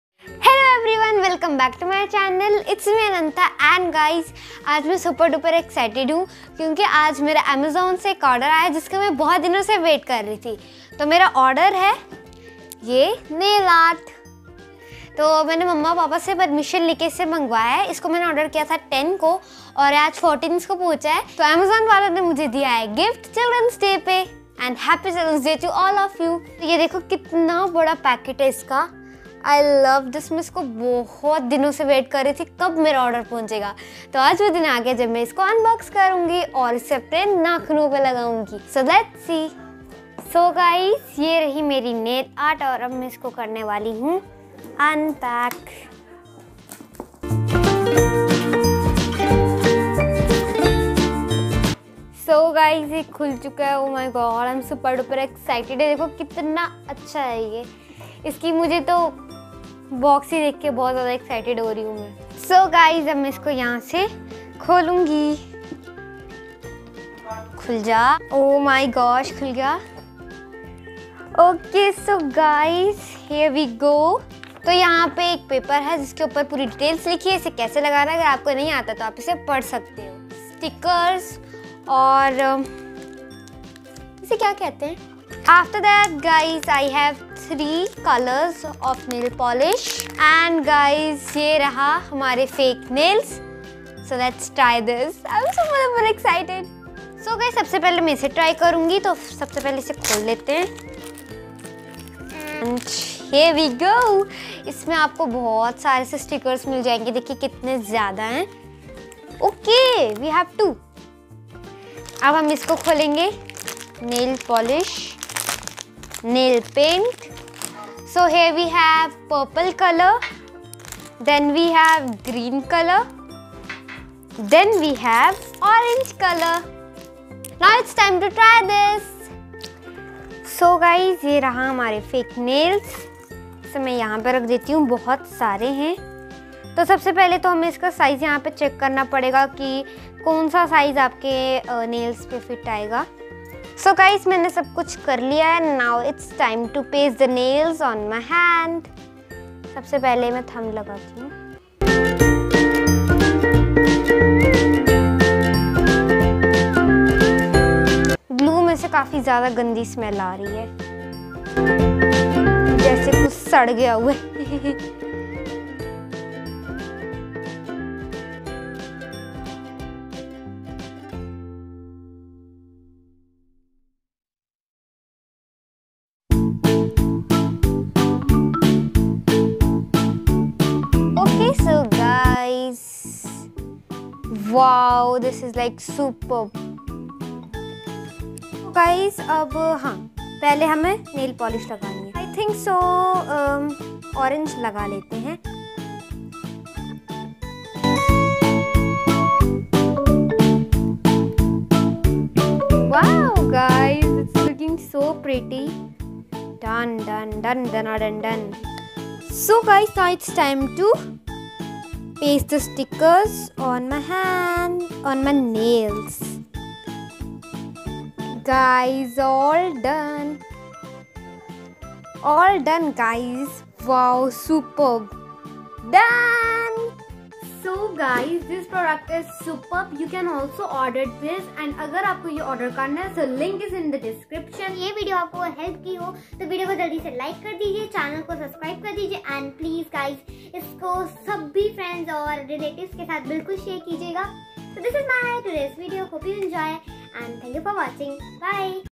Unboxing my new Nail Art Studio Kit | Video on Children's Day
Unboxing my new Nail Art Studio
 Kit | Video on Children's Day

Here is the Link. It's not any promotion!